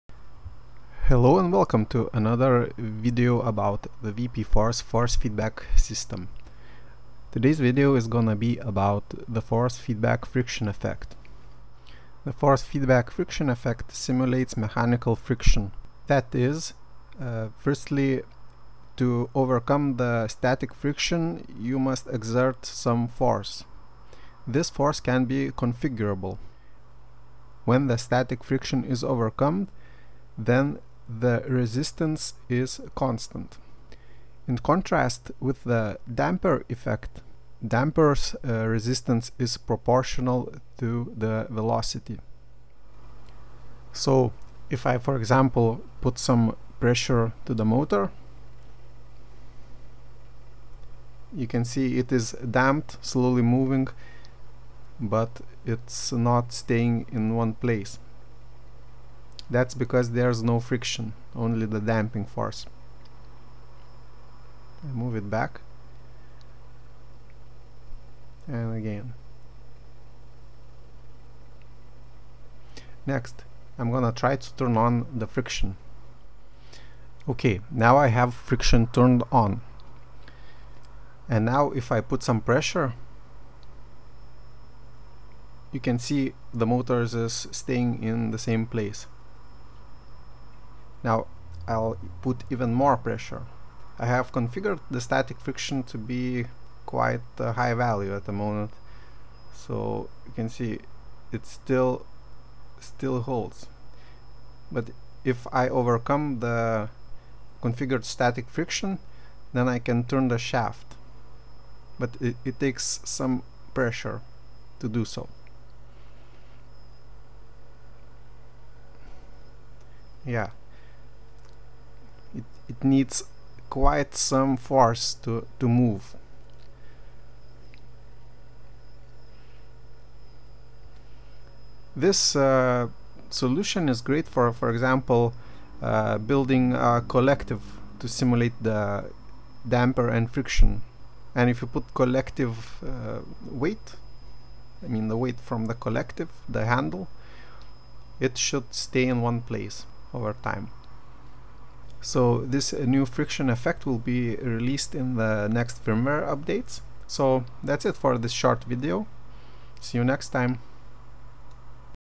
VPforce FFB New Friction Effect Development
New research on how implement FFB friction effect. The new effect properly simulates static friction.
This system will enable enthusiasts to build a plug-an-play FFB joysticks with custom mechanics.